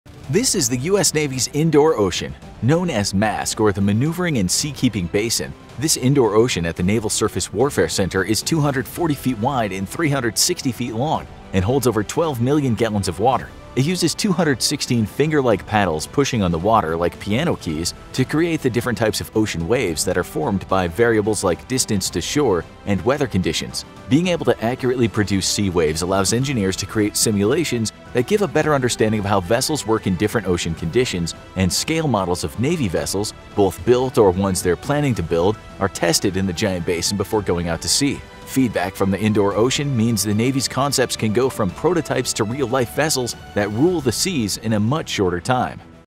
The Navy Built an Indoor Ocean
THIS is the US Navy’s Indoor Ocean.
Known as MASK, or the Maneuvering and Seakeeping Basin, this indoor ocean at the Naval Surface Warfare Center is 240 feet wide and 360 feet long and holds over 12 million gallons of water.
It uses 216 finger-like paddles pushing on the water like piano keys, to create the different types of ocean waves that are formed by variables like distance to shore and weather conditions.
Being able to accurately produce sea waves allows engineers to create simulations that give a better understanding of how vessels work in different ocean conditions, and scale models of Navy vessels - both built or ones they are planning to build- are tested in the giant basin before going out to sea.
Feedback from their indoor ocean means the Navy’s concepts can go from prototypes to real-life vessels that rule the seas in a much shorter time.